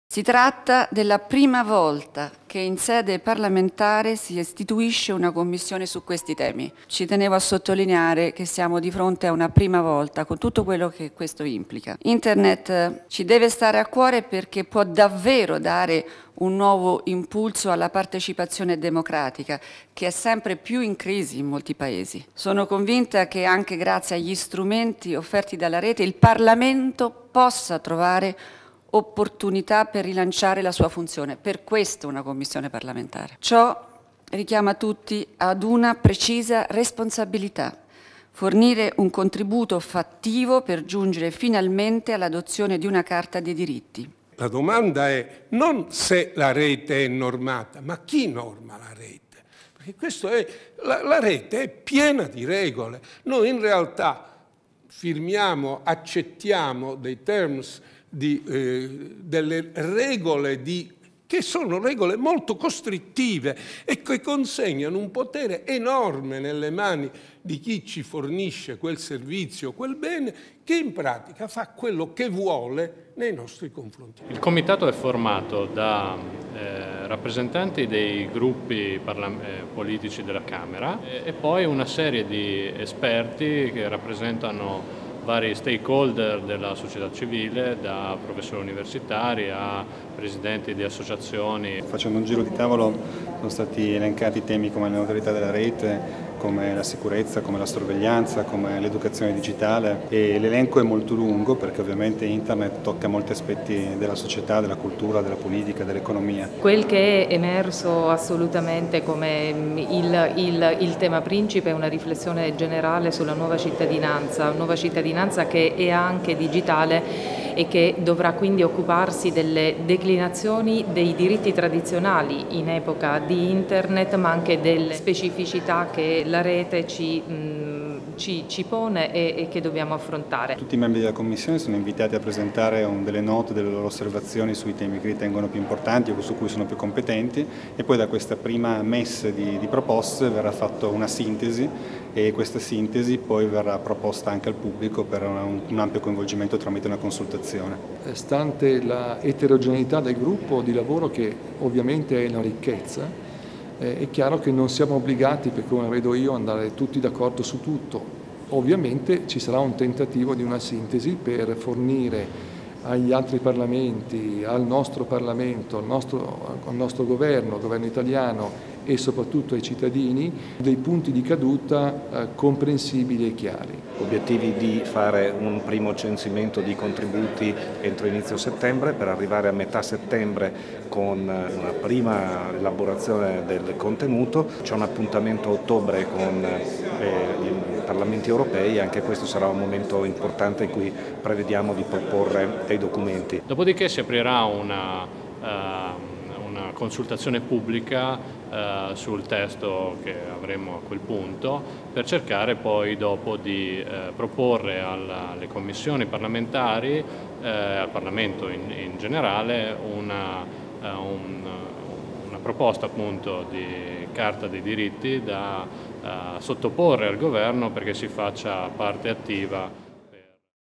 Al via la Commissione per i diritti e doveri relativi ad Internet
Si è tenuta oggi la prima seduta della nuova Commissione di studio per i diritti e doveri relativi ad Internet, promossa dalla Presidente della Camera dei deputati Laura Boldrini, composta da 23 membri fra deputati, esperti del settore, rappresentanti delle imprese, delle associazioni e della società civile, tutti partecipanti a titolo gratuito.

"Si tratta della prima volta che in sede parlamentare si decide di istituire una Commissione su questi temi; è una esperienza di grande valore che mi auguro possa portare a risultati concreti e significativi", ha dichiarato la Presidente Boldrini nella sua introduzione.

I primi risultati saranno sottoposti all'attenzione dei partecipanti alla riunione dei Parlamenti dei Paesi membri dell'Unione europea e del Parlamento europeo sui diritti fondamentali che si terrà presso la Camera il 13 e il 14 ottobre 2014 nel corso del semestre di Presidenza dell'Unione europea. Contemporaneamente, le proposte che la Commissione elaborerà saranno sottoposte ad una consultazione pubblica per assicurare una partecipazione più larga possibile alla loro individuazione.

Nel video alcuni degli esperti e dei parlamentari membri ricostruiscono la prima riunione di lavoro.